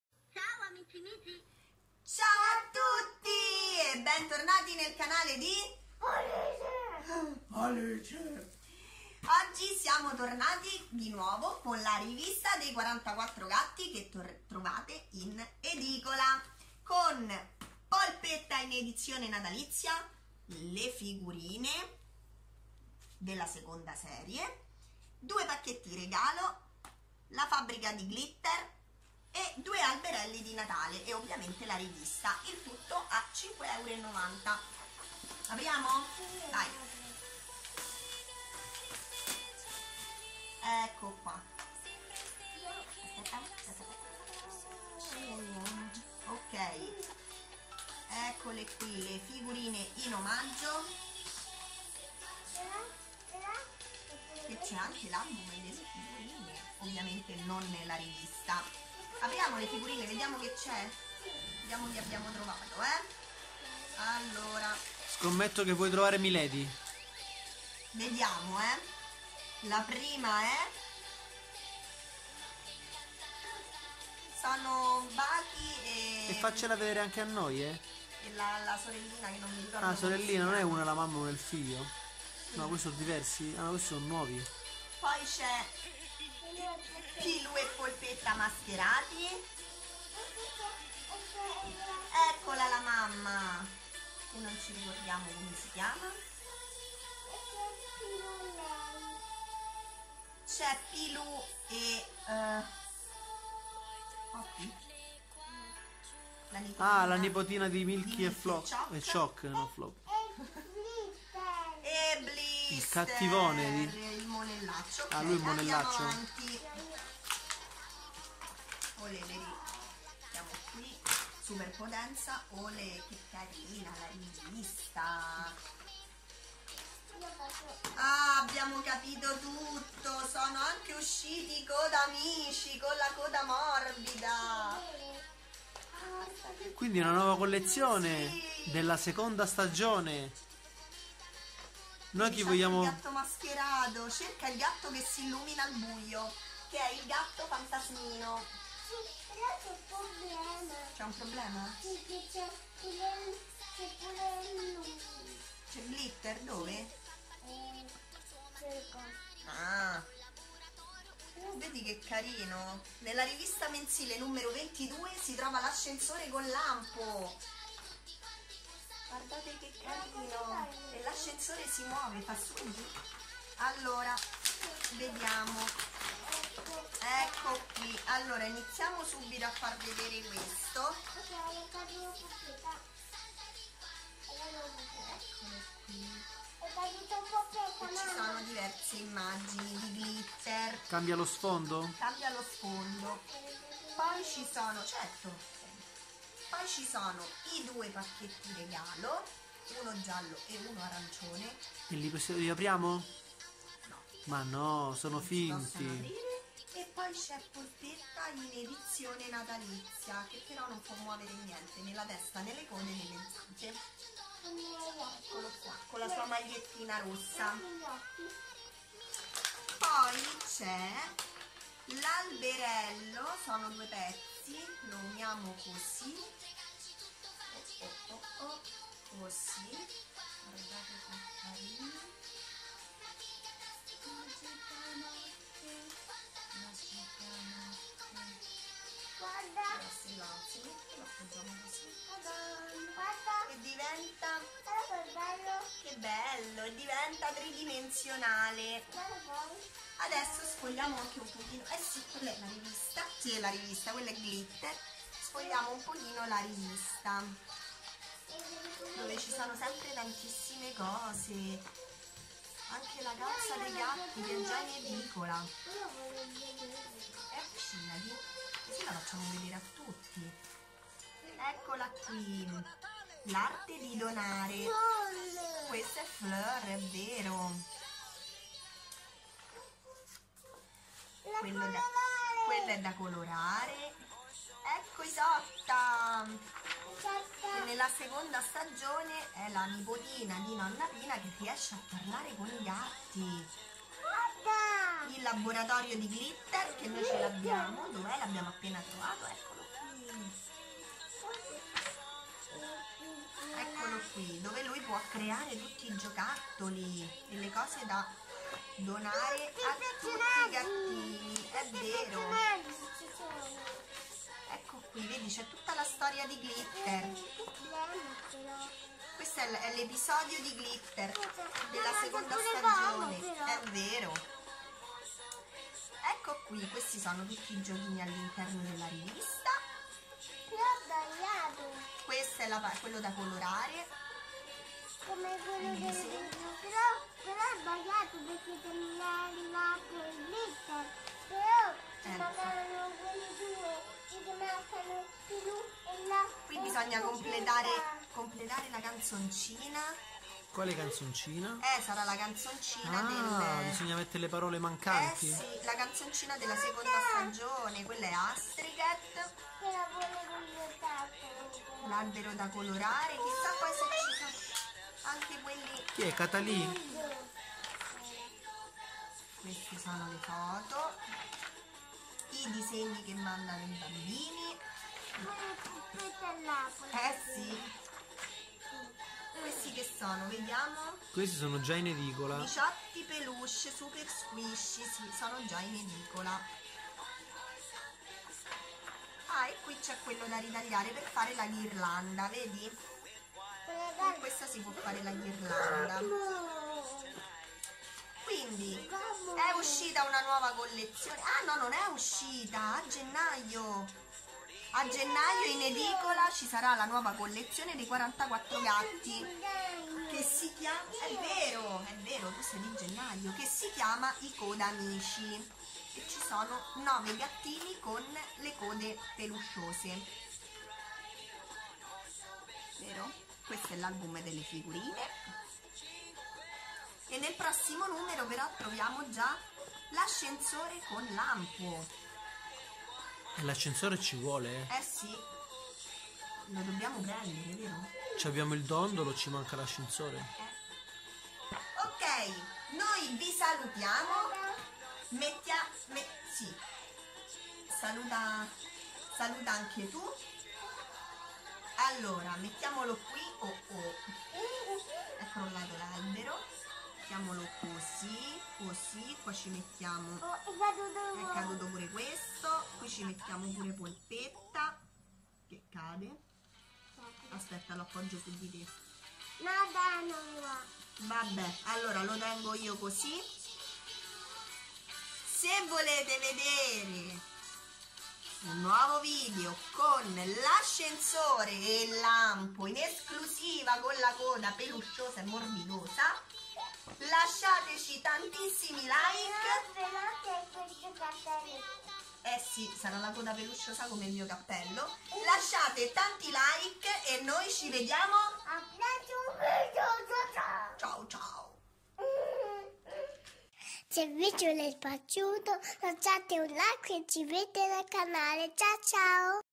🐱🐈 44 Gatti 😻 😺🎸 Il magazine della serie TV 🌲😼😽🌲 Edizione Natalizia 😽😹😻
Bentornati sul nostro canale, oggi apriremo la rivista dei 44 gatti che potete trovare in edicola insieme a polpetta in edizione natalizia.. Correte a prenderli